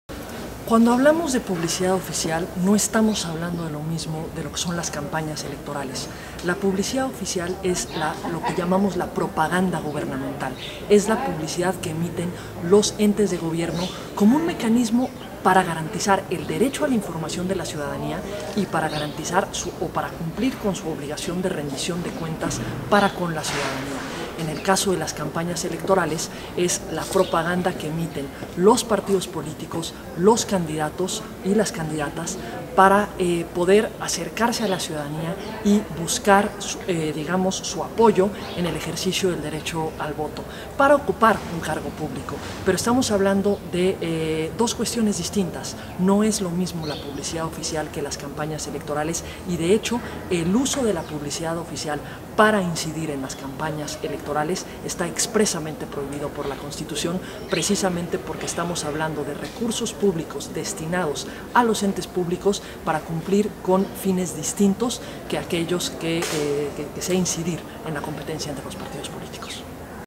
La publicidad oficial no es propaganda electoral: Pamela San Martín
La publicidad oficial debe funcionar como un mecanismo de los gobiernos para garantizar el derecho a la información de la sociedad: Pamela San Martín, consejera del INE, en el Foro MiLanaNoEsMordaza.